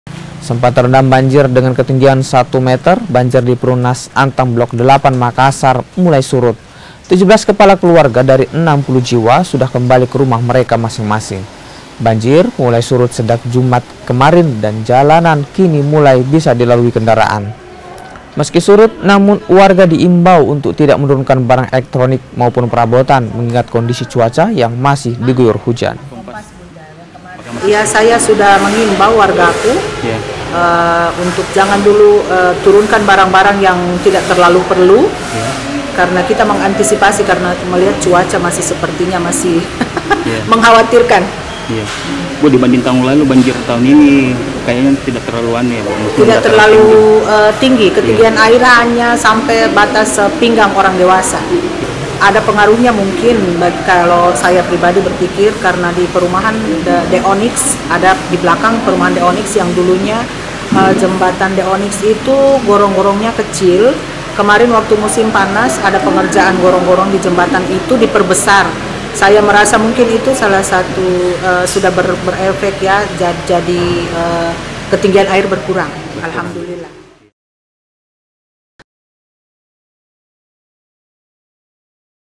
Banjir Surut, Pengungsi Kembali Ke Rumah
MAKASSAR, KOMPAS.TV - Banjir yang merendam perumnas antang blok 8, kecamatan Manggala, kota Makassar, Puluhan warga yang sebelumnya mengungsi, kini kembali kerumah mereka.

Sempat terendam banjir dengan ketinggian satu meter, banjir diperumnas antang blok 8, Makassar, kini surut. 17 kepala keluarga dari 60 jiwa, sudah kembali kerumah mereka.

Banjir mulai surut sejak jumat malam, dan jalanan kini mulai bisa di lalui kendaraan.

Meski surut, namun warga di imbau untuk tidak menurungkan barang elektronik maupun perabot, mengingat kondisi cuaca yang masih diguyur hujan.